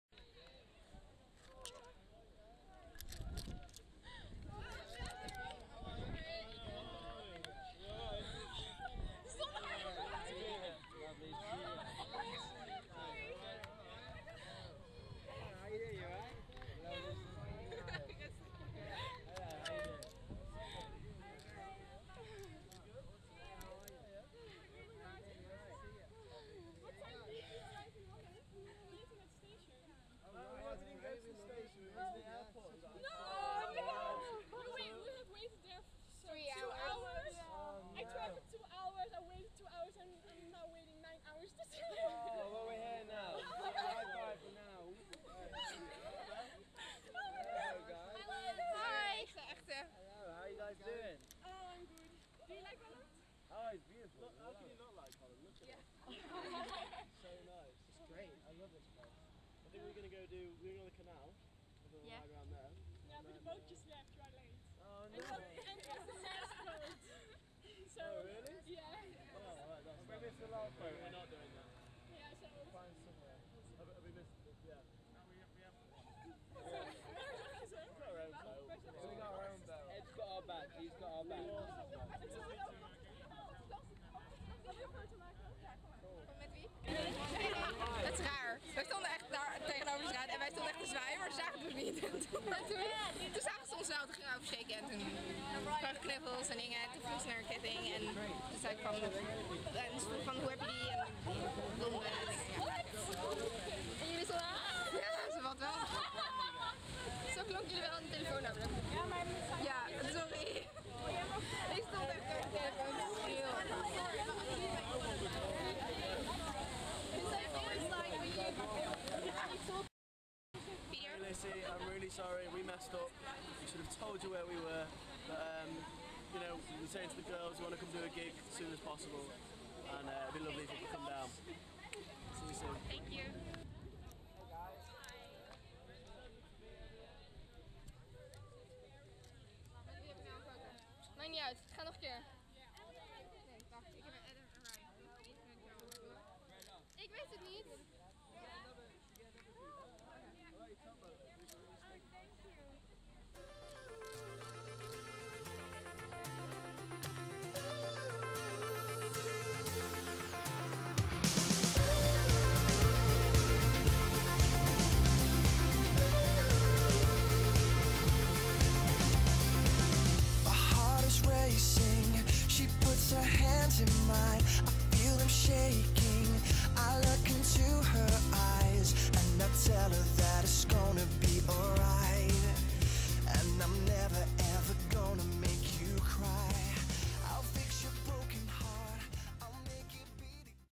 Meeting lawson
I met Lawson when they were in Amsterdam last Tuesday (:
The whole day I hoped I could hug Joel Peat, the first thing that happened when we finally met them (after we waited for 5 hours) was Joel hugging me :D Adam hugged me aswell, I was so happy! I've took a picture with all of them!(:
At the end of the video, you see me with all the Lawson members and also my friend Gabrielle (:
Sorry for the bad quality of the sound, I had my hand on the mic half the time, so it's not equal ;)
Prepare your ears!